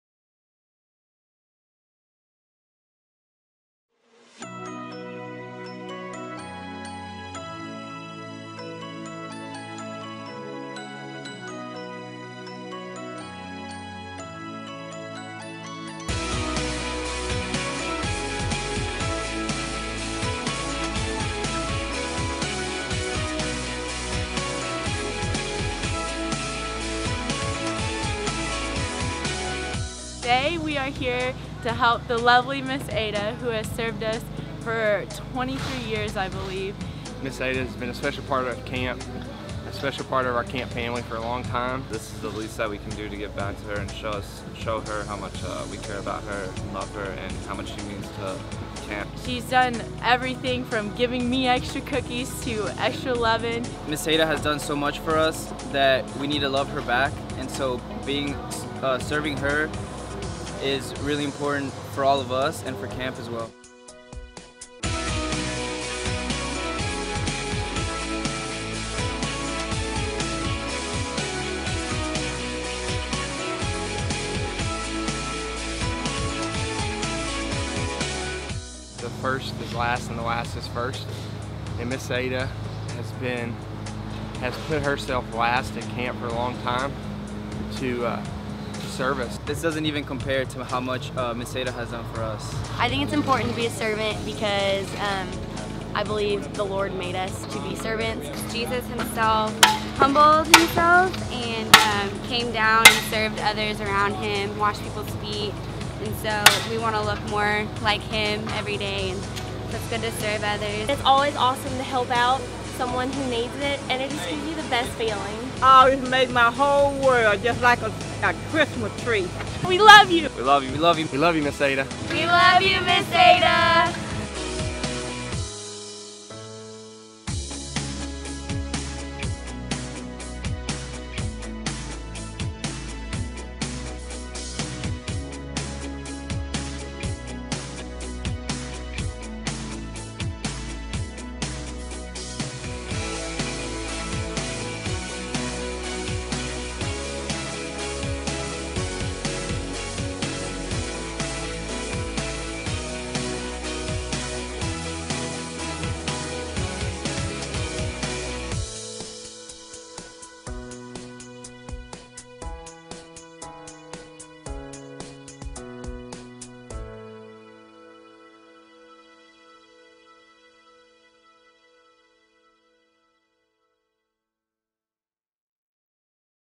Serving Miss Ada | Camp Huawni 2014
The lovely Ms. Ada has been serving Camp Huawni for 23 years. Now it is time to give back and serve her in order to thank her for all she's done.